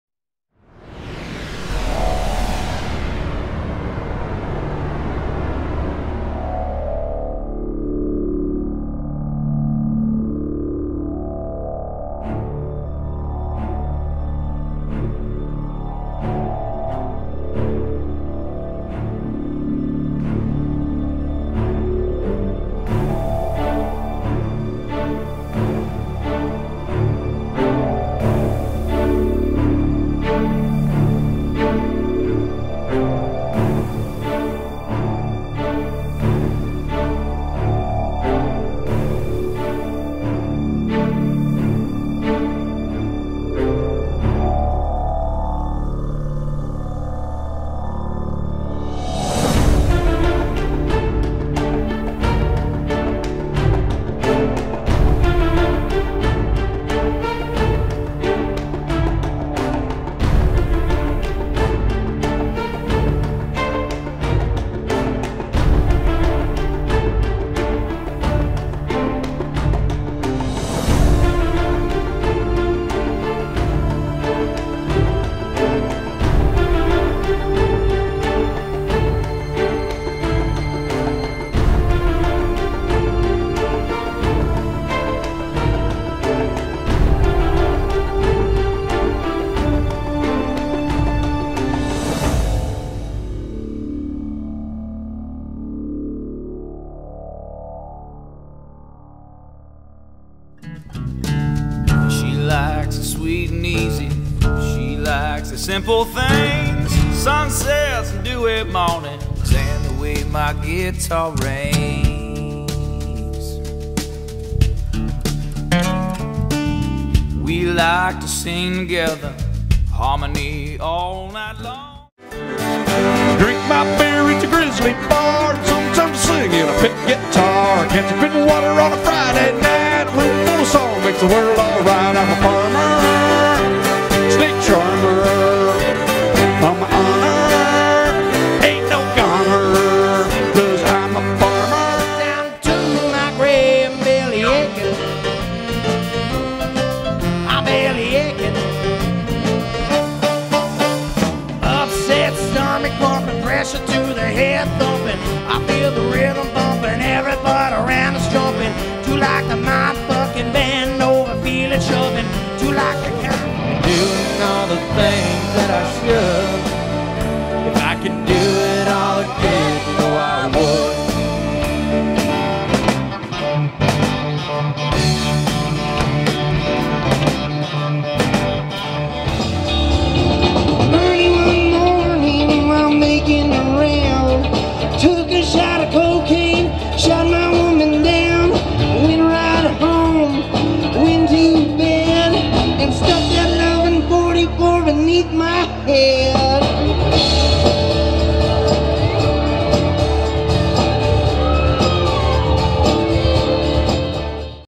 Wheatstock 2011
Get your tickets now for this years Wheatsock Music Festival in Helix, Oregon on Saturday August 20, 2011. Headliner this year is Stoney Laurue from Texas!!